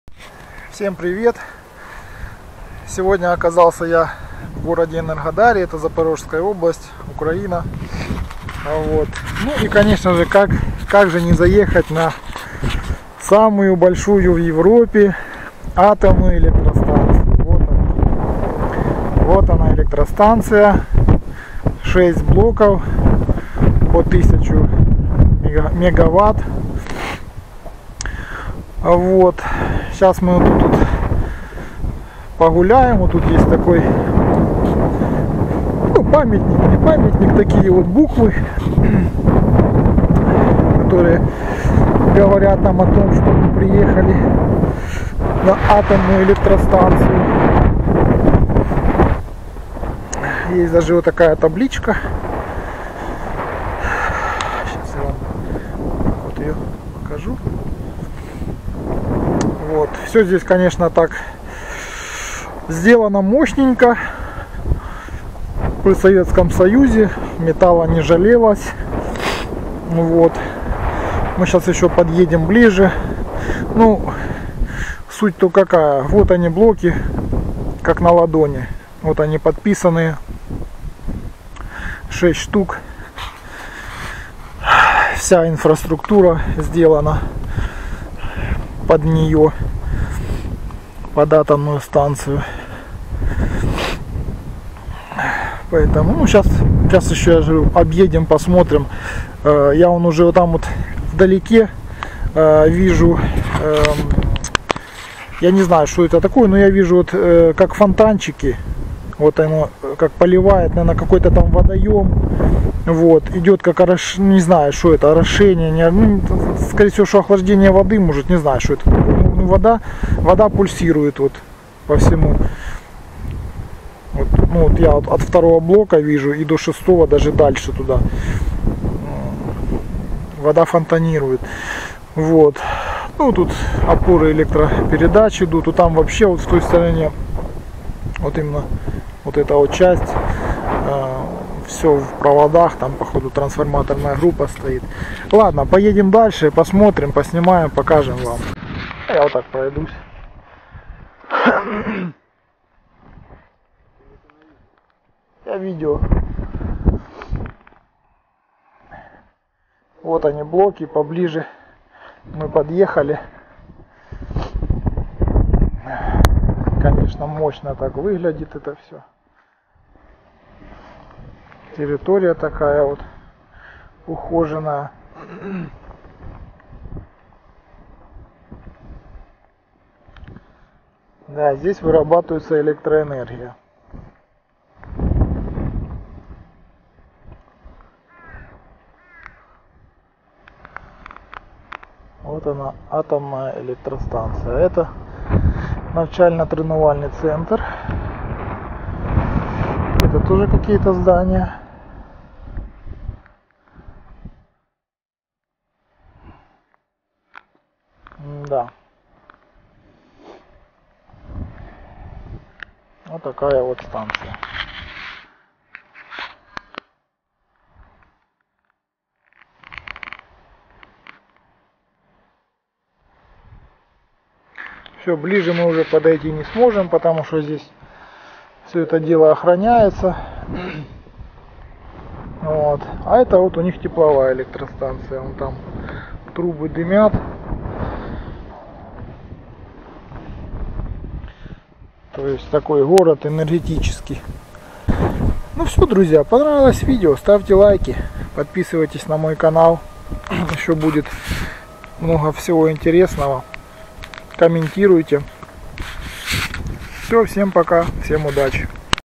Энергодар - Запорожская АЭС 2018
Энергодар - Запорожская АЭС 2018. Небольшая видеоэкскурсия возле атомной электростанции которая находится в городе Энергодар Запорожской области